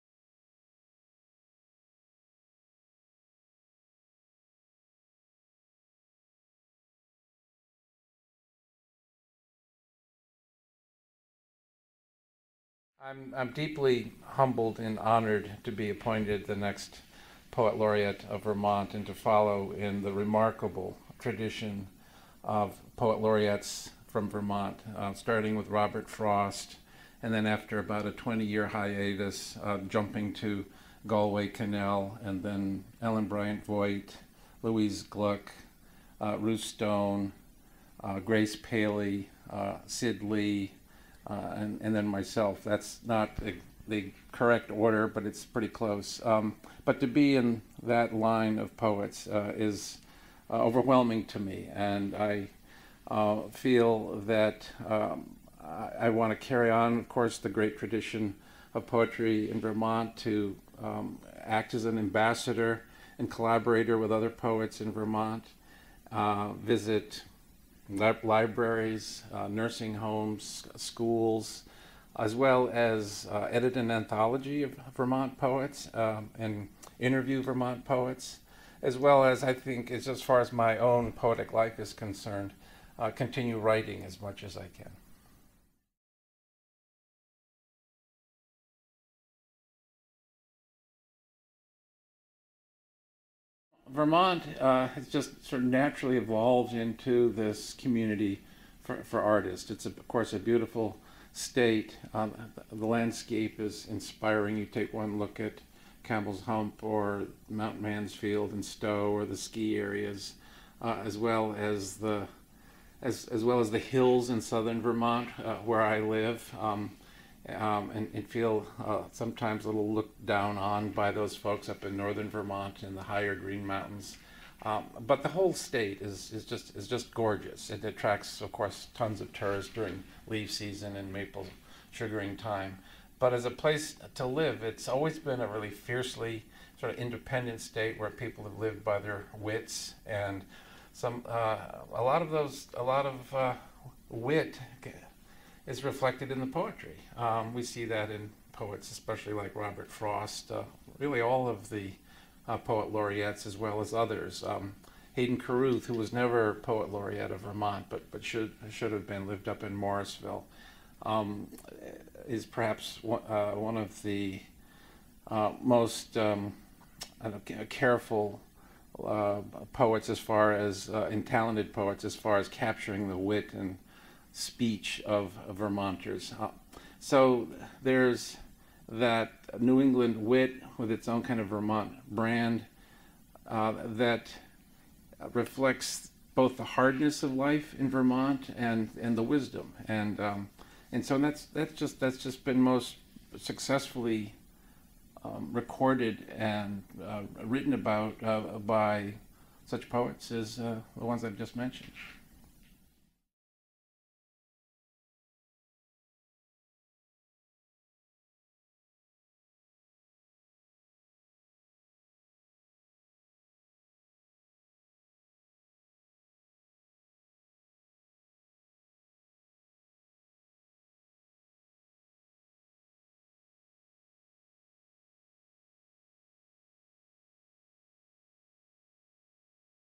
Chard deNiord: Vermont's Poet Laureate 2015-2019
In this video, Chard deNiord shares his thoughts about becoming Poet Laureate of Vermont.